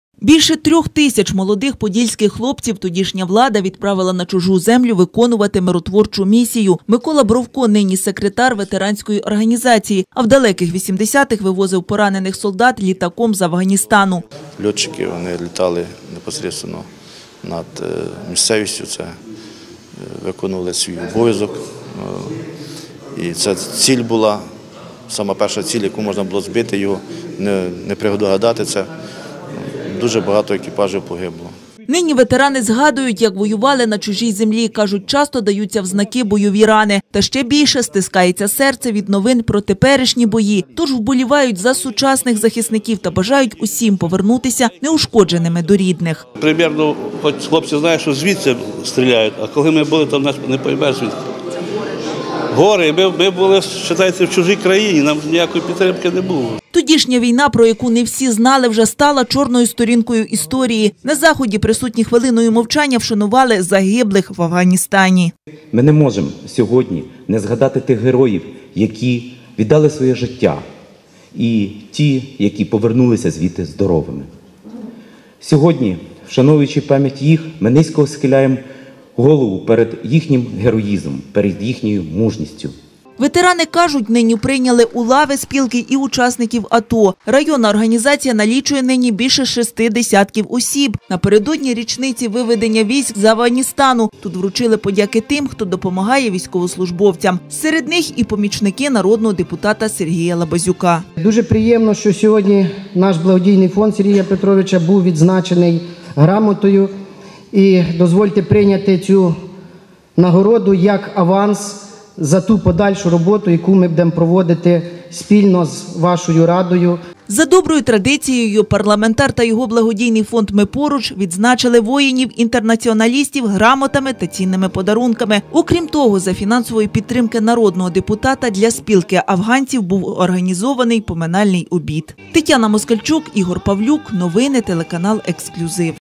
Воїни-афганці вшанували полеглих побратимів
Відлуння війни. Напередодні річниці виведення миротворчих військ з Афганістану, учасників усіх локальних конфліктів вшанували в Хмельницькому районі. Ветерани згадали полеглих побратимів та побажали  бійцям на сході країни перемоги над ворогом.